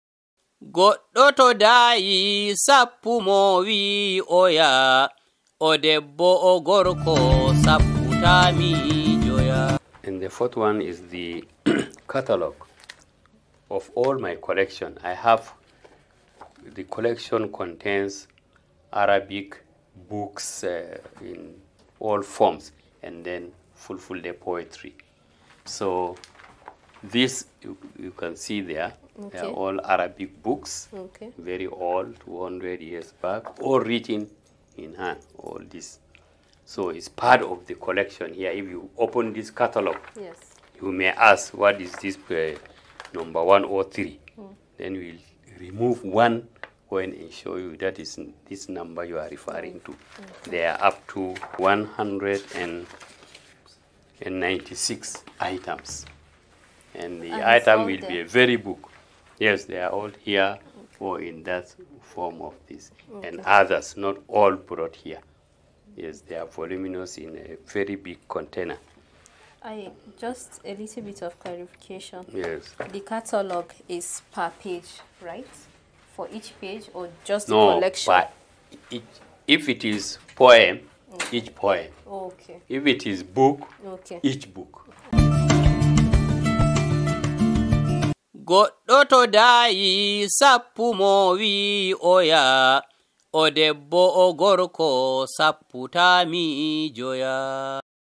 E3: MBA Collection Catalogue
I am excited to introduce you to the 13-Episode Series Exhibition (A corpus of transliterated 19th century Arabic/Ajami Fulfulde Poetry and contemporary composition of Fulfulde Poems) that introduces MBA Collection.

MBA (Muhammad Bashir Abubakar) is the wonderful person behind it and he has spent over 5 decades working on these collections.

Filmed by: Ahmed Umar

Questioner: Fatima Binta Bashir

Exhibitor: Muhammad Bashir Abubakar

Edited by: IamAbubakarBash

If you have any enquiries or want to be part of this amazing project, do not hesitate to reach out.